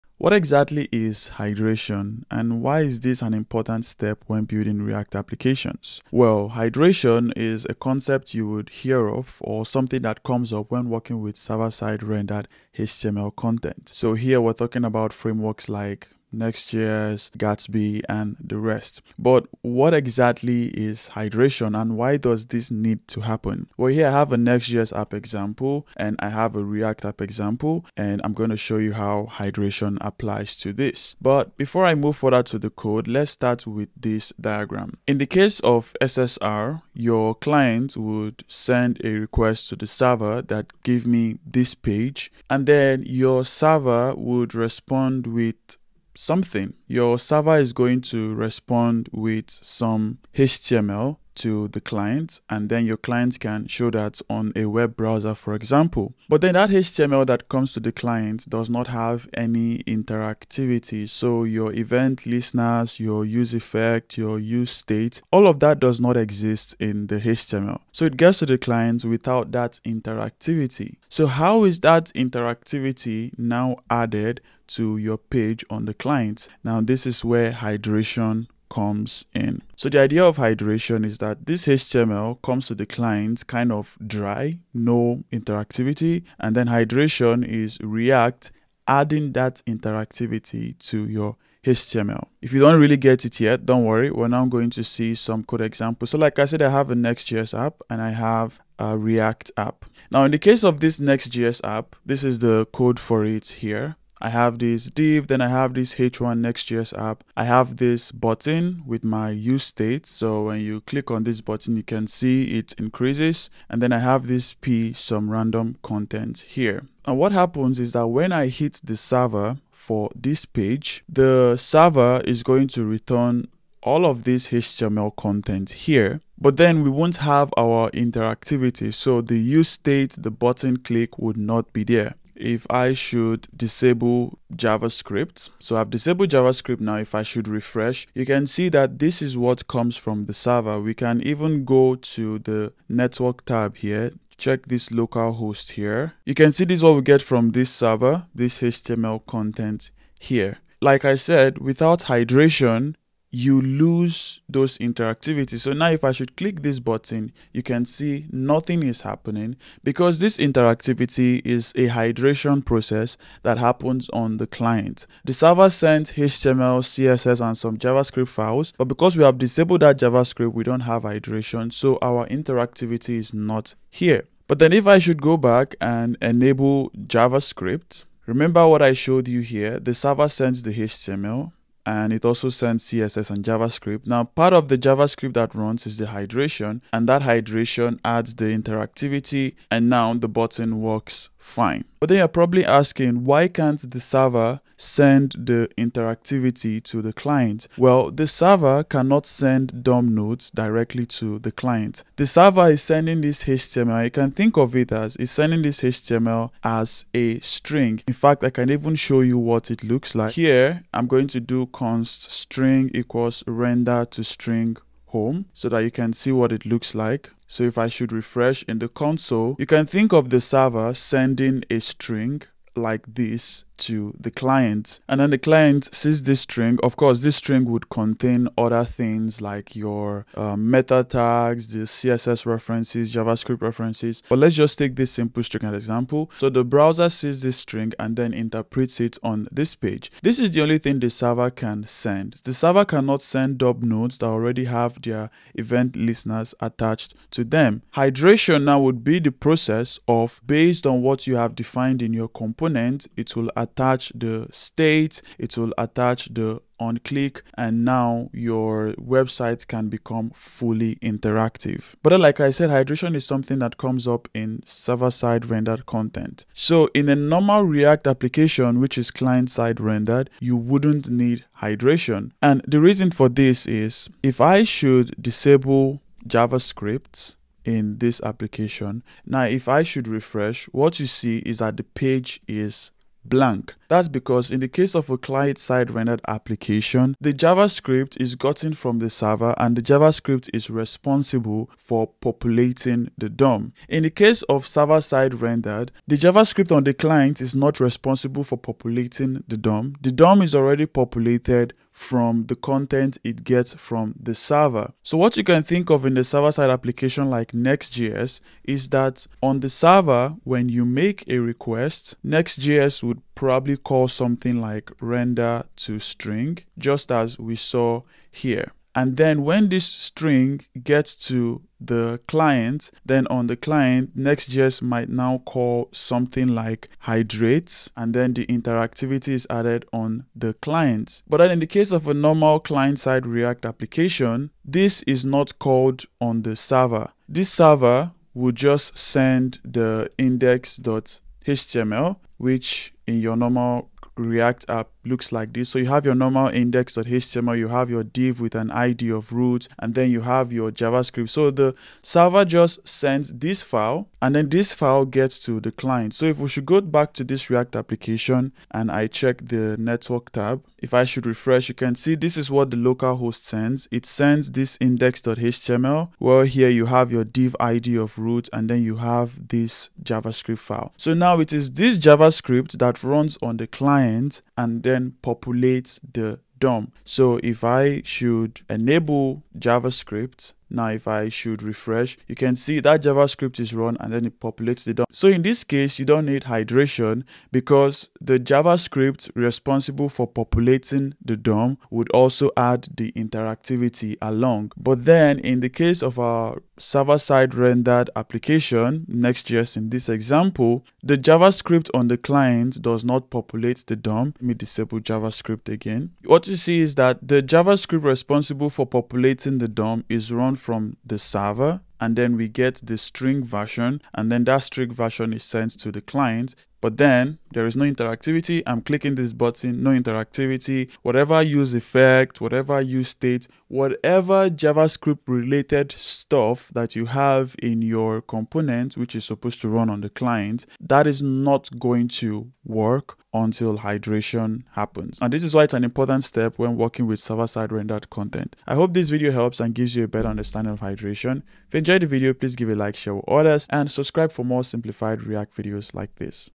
What Exactly is REACT HYDRATION? And why does it matter?
If you've been writing React for a while, or using React-based frameworks like NextJS and GatsbyJS, you may have heard of hydration. But what exactly does this mean? I explain that in this video.